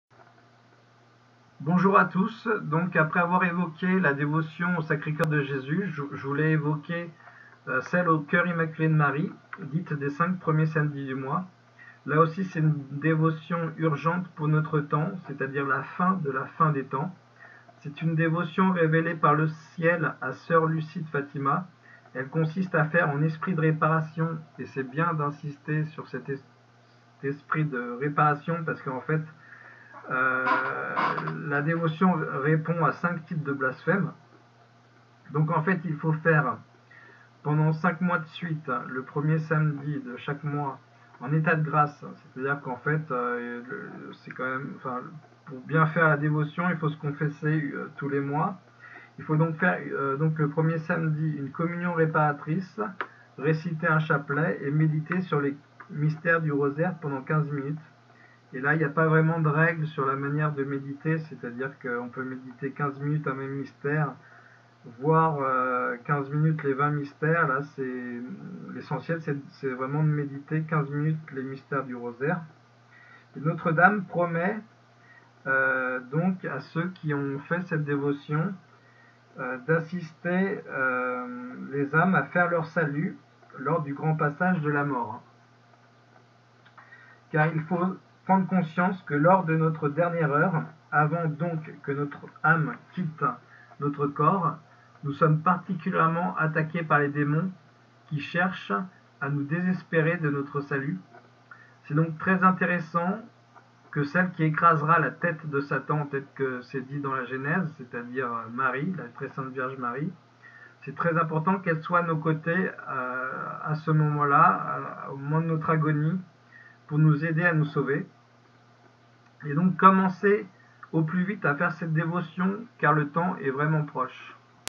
Assurer Son Salut par le Coeur Immaculé (la dévotion aux 5 premiers du mois)
La dévotion aux 5 premiers samedis du mois :

Notre-Dame à sœur Lucie de Fatima le 10 décembre 1925 à Pontevedra en Espagne :
« Tous ceux qui, pendant cinq mois, le premier samedi, se confesseront, (ou 8 jours après ou avant, l'essentiel est d'être en état de grâce) recevront la sainte Communion, réciteront un chapelet, et me tiendront compagnie pendant quinze minutes en méditant sur les quinze mystères du Rosaire,  en esprit de réparation, je promets de les assister à l'heure de la mort avec toutes les grâces nécessaires pour le salut de leur âme.»

🎁Pour un don en Bitcoin (BTC) :
1EDkdEqg1yCEXRyezortD5xUFAugH3a3Gc